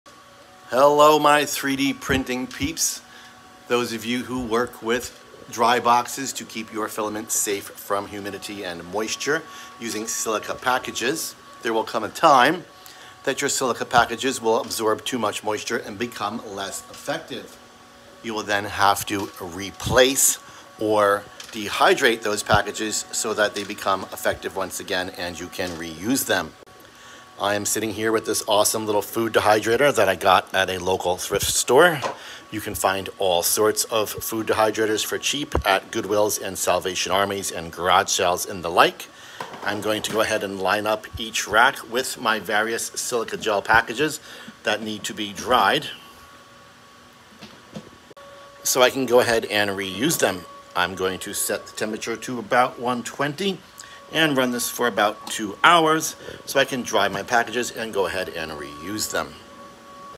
Dry and Reuse your Silica Packs, for your 3D Printer Filament Dry Box, using a food Dehydrator
Dry & Dry
Dry Tote